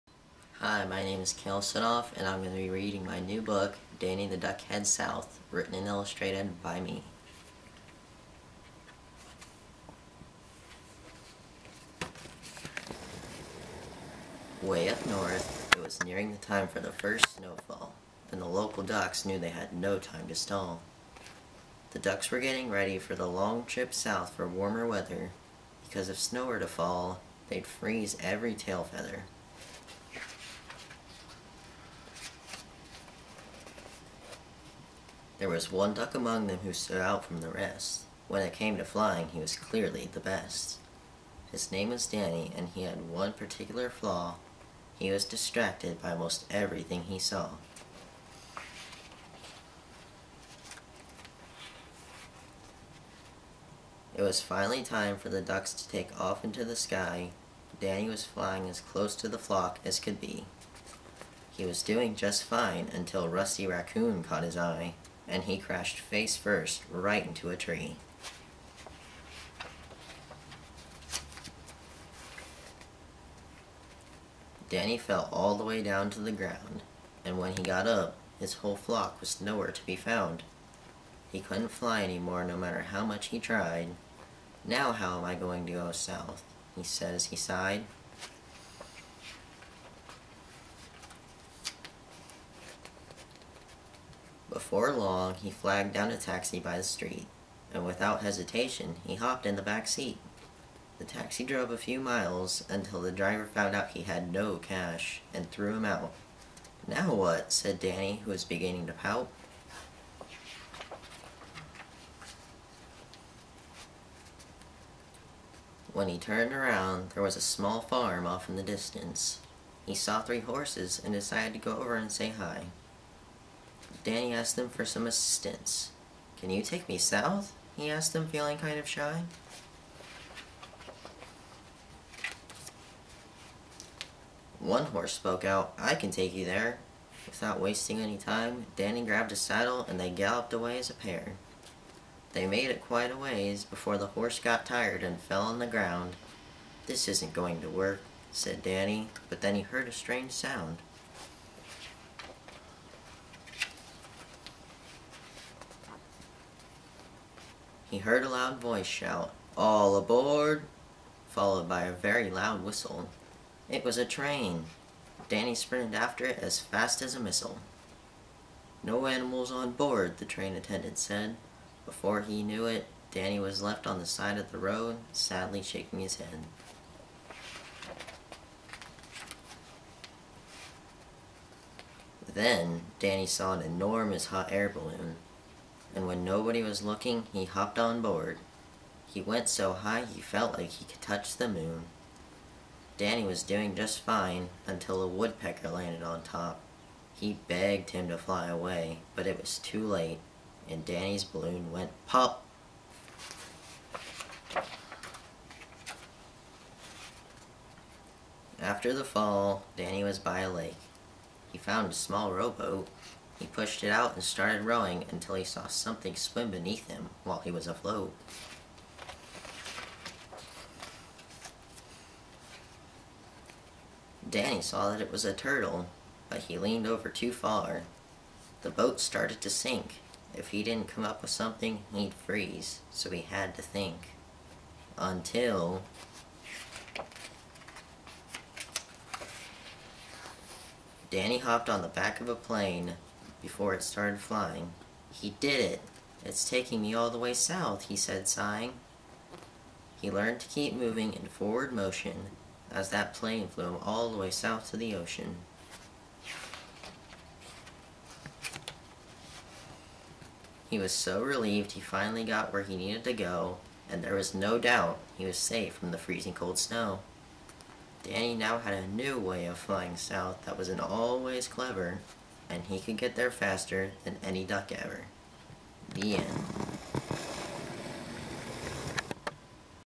Kale Sudhoff reading Danny the Duck Heads South
Kale is reading his new book "Danny the Duck Heads South" that he recently wrote and illustrated.